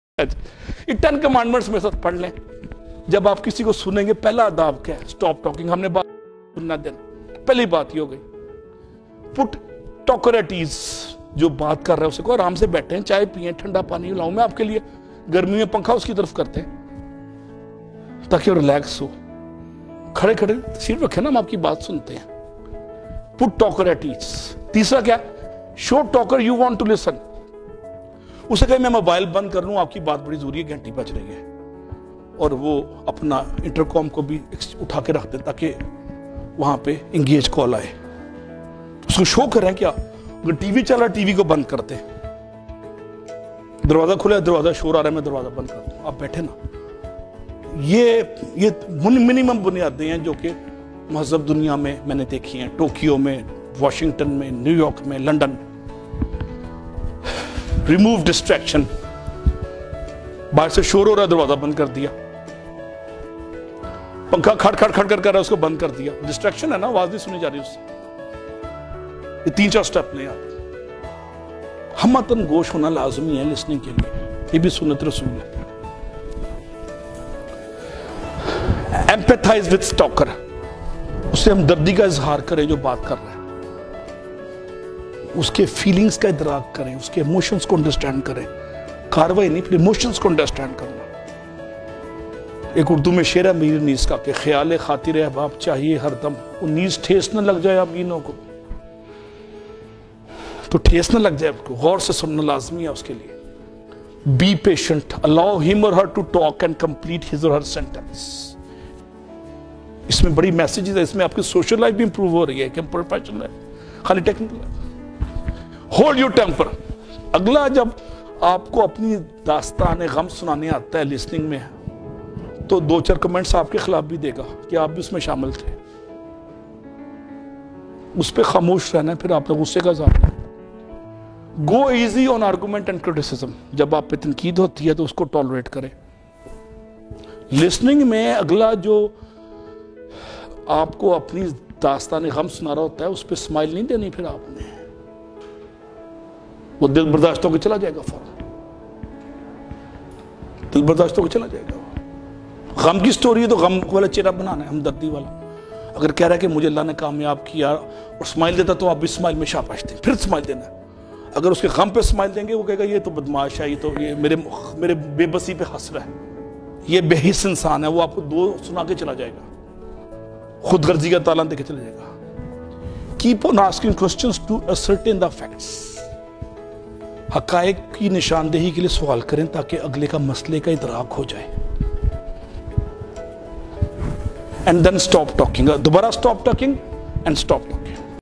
The Ten Commandments of Listening
Dr. Ali Sajid (Director of LSM, LSPS and LMDA) is delivering lecture on "The Ten Commandments of Listening".

Lahore School of Management
94 College Block, Near Science College, Wahdat Road Lahore